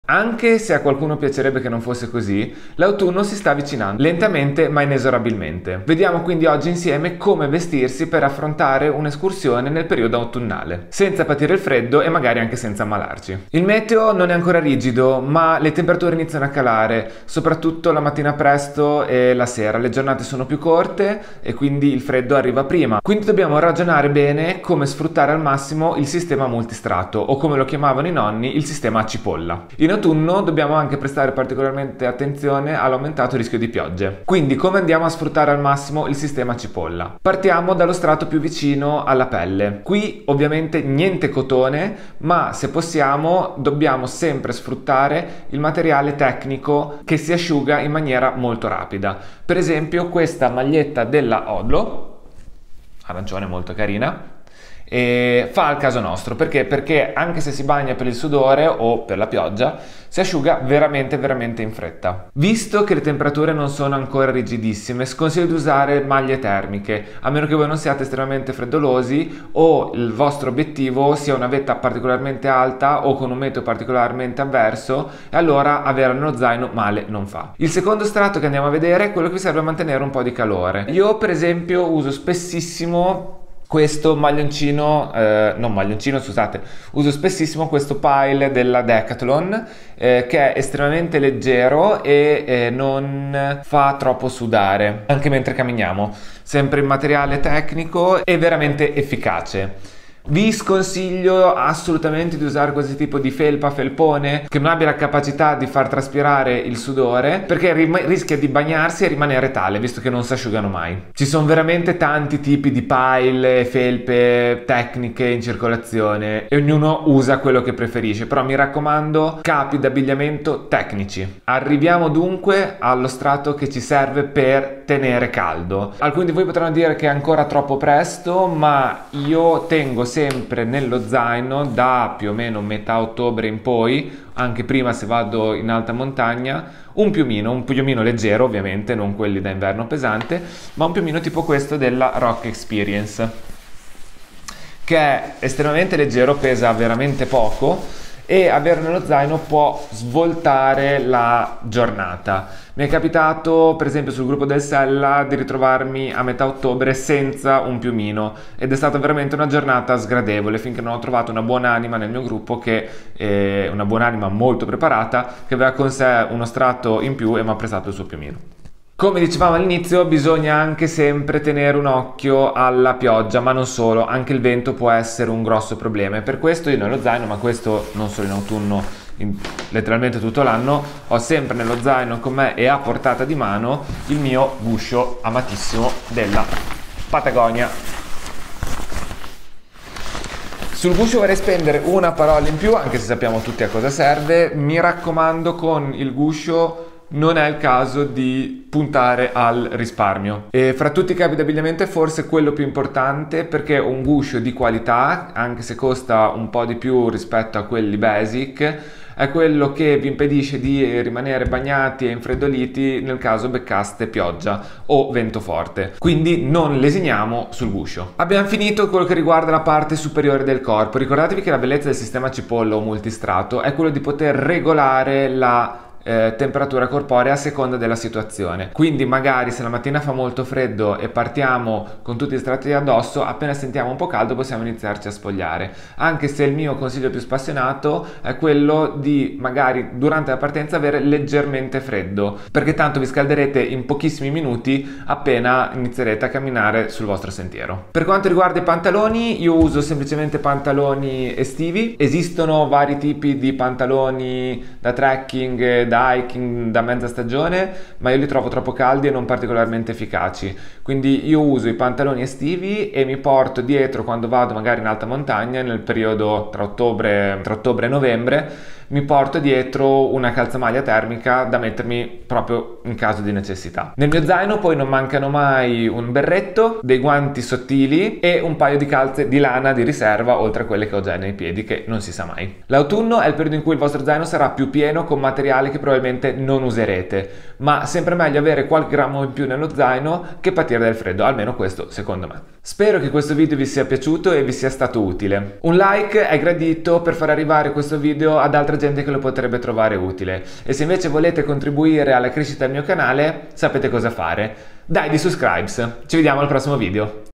Come vestirsi per un escursione in autunno in montagna
L'autunno è alle porte ma non vogliamo rinunciare alle nostre escursioni. Ecco quindi che in questo video scopriamo insieme come vestirsi al meglio per godersi una giornata per sentieri in autunno!

Per domande su come vestirsi per un'escursione autunnale lasciate un commento!